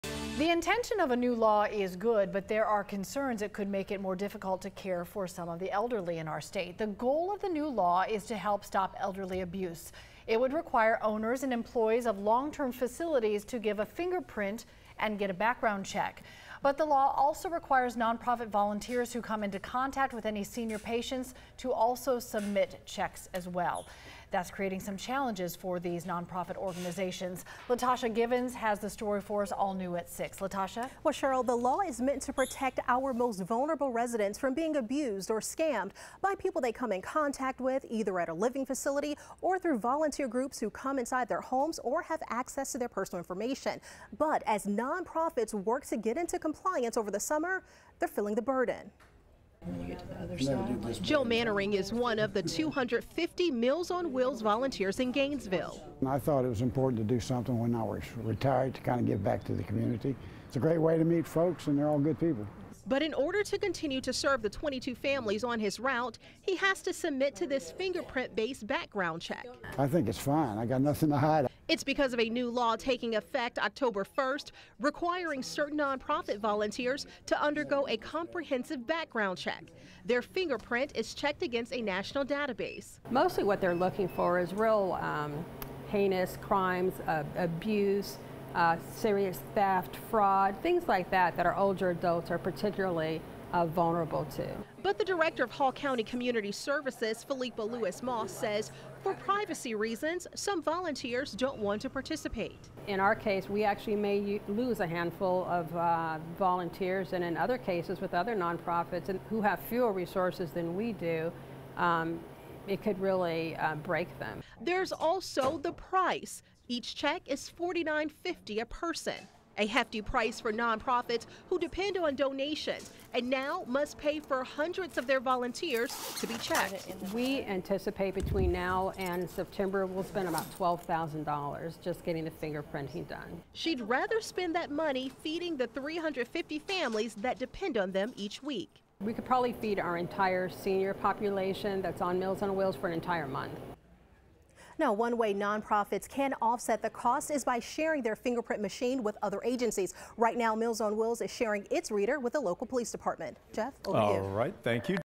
New law could make it difficult to care for elderly
The goal is to stop elderly abuse. However, there are concerns.